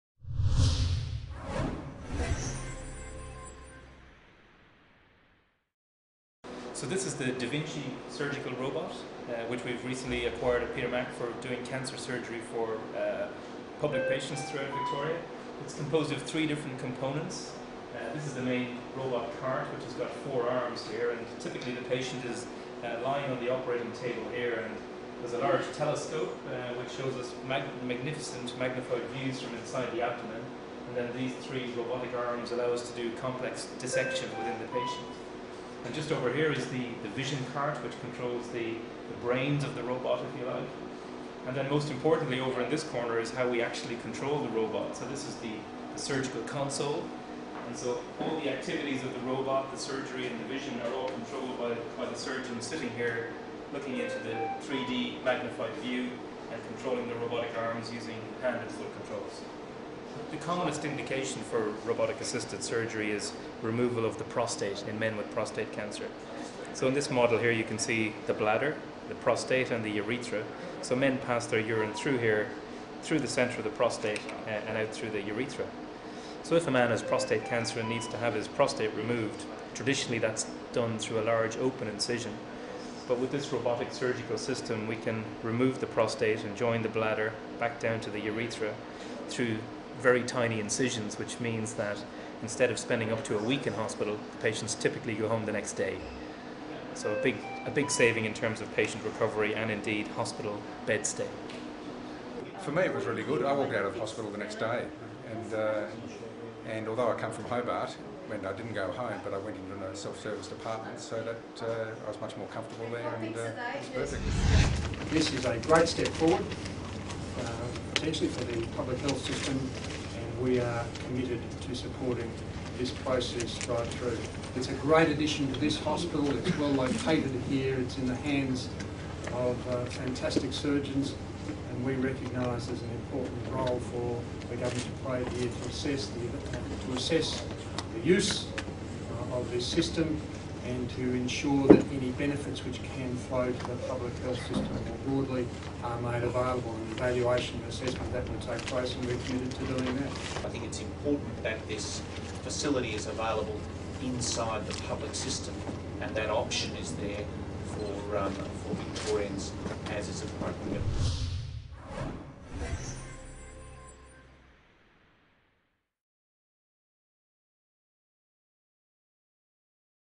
Da Vinci S HD Robot at Peter MacCallum Cancer Centre.mov
May 2011 - Premier Ted Bailieu and Minister for Health David Davies visit the Peter MacCallum Cancer Centre to launch the Academic Robotic Cancer Surgery Program.